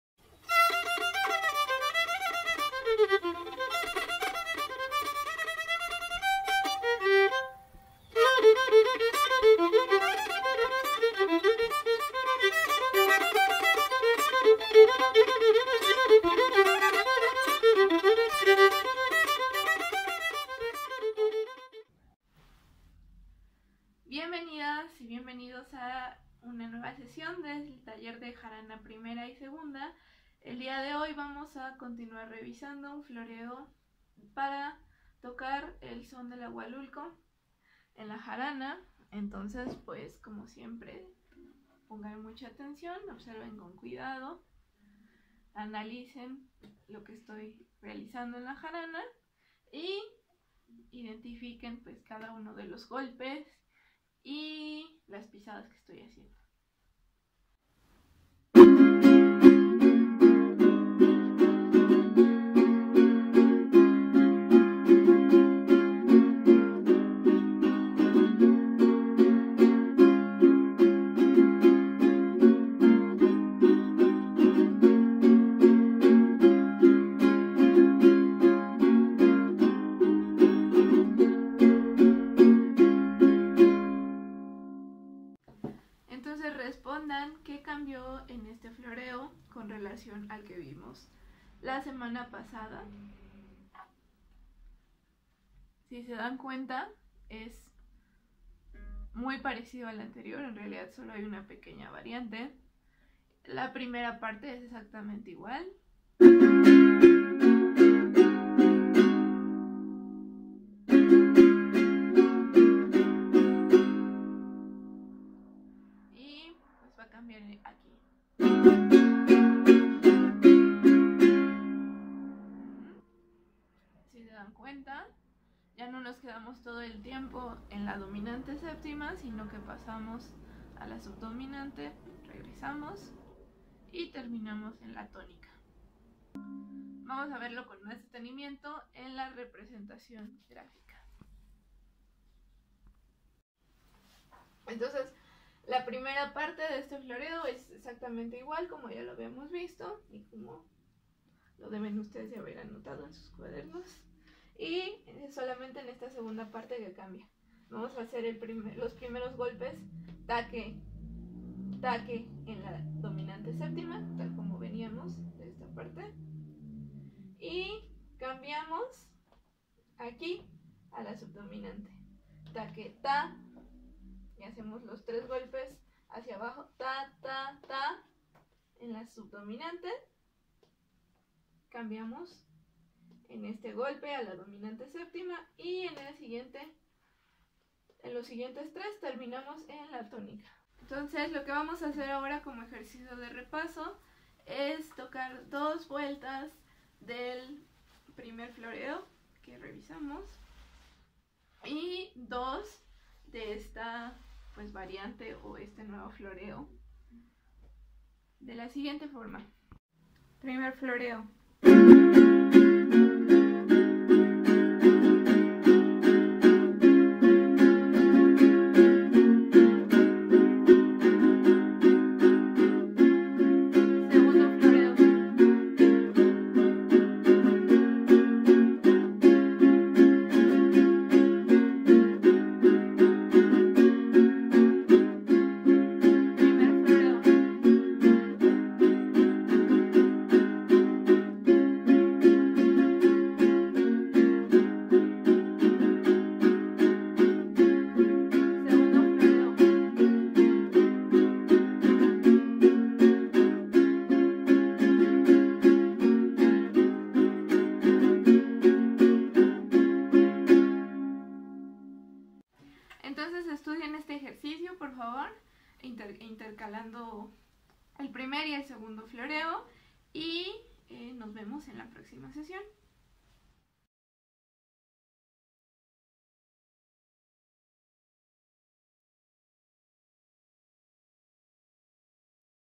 Taller de jarana 1a y 2a | SEMBRANDO MUSICA | CCC | Decimotercera sesión
Decimotercera sesión del taller de música tradicional del proyecto Sembrando Música, como parte del Programa Colectivos Culturales Comunitarios de la Secretaria de Cultura CDMX.